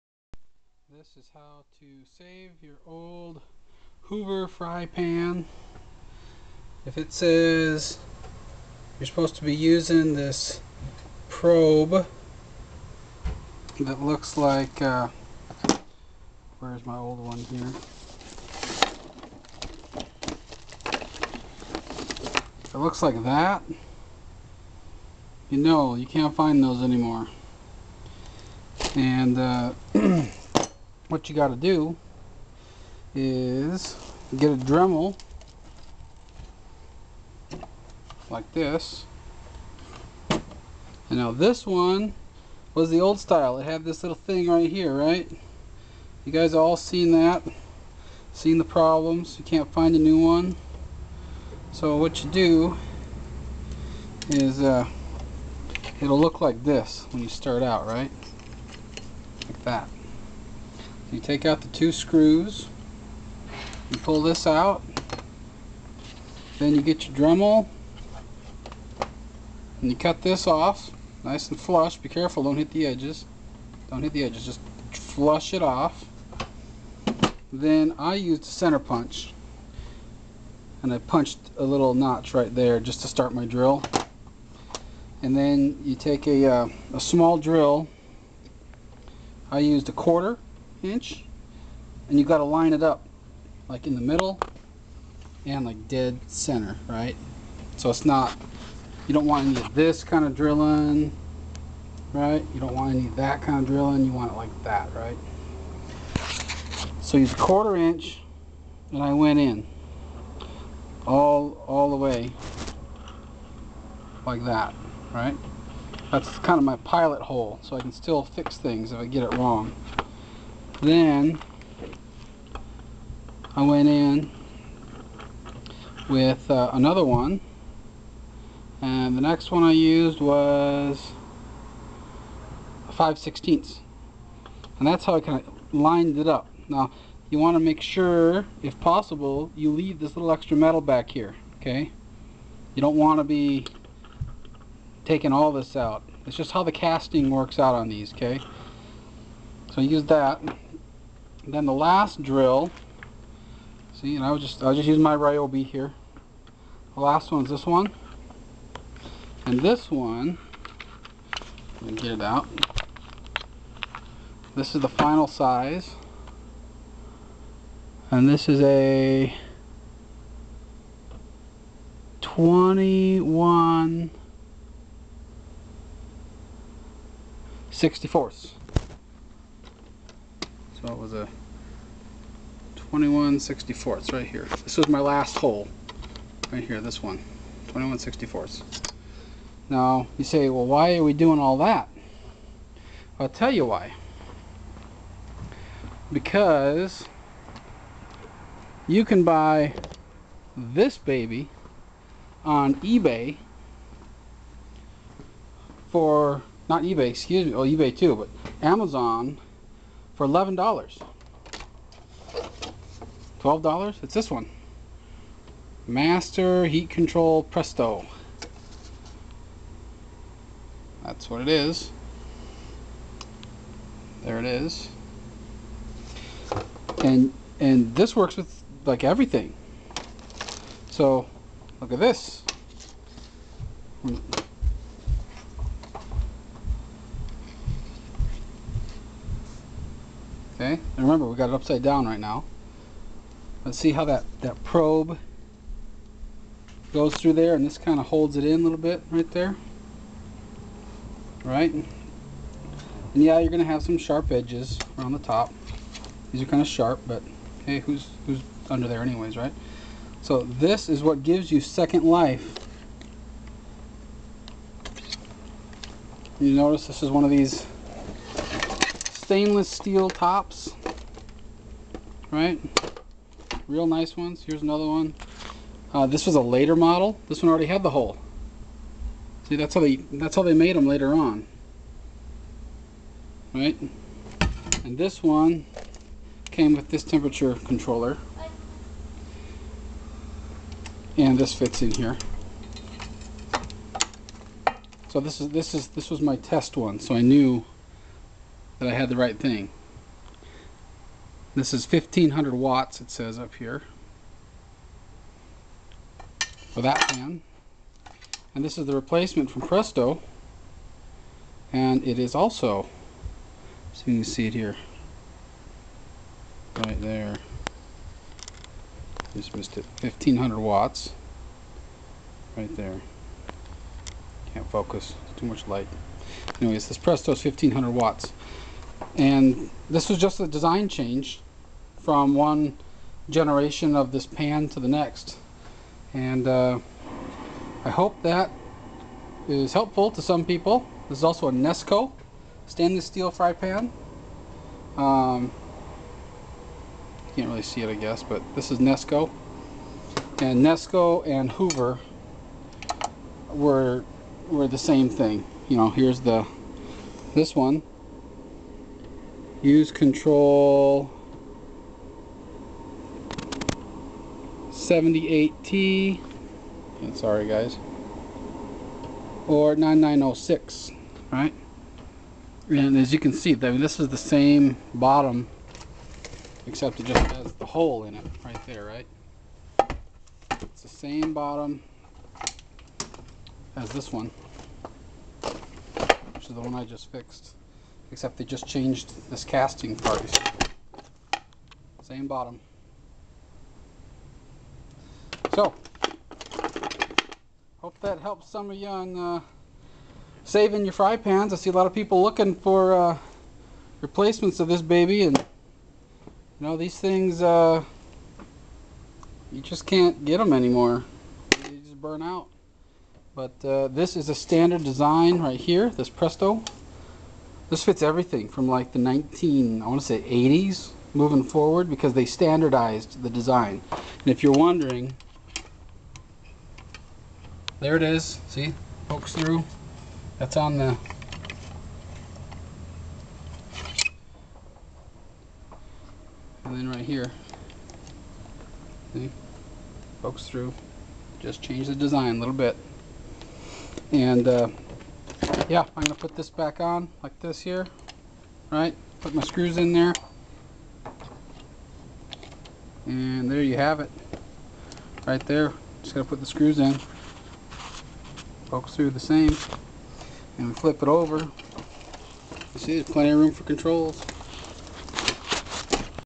Hoover Nesco Stainless Skillet Cord Fix Repair
These cords are available anymore, but you can still save your old stainless Nesco Hoover Skillet. This is how I fixed mine and it works like new.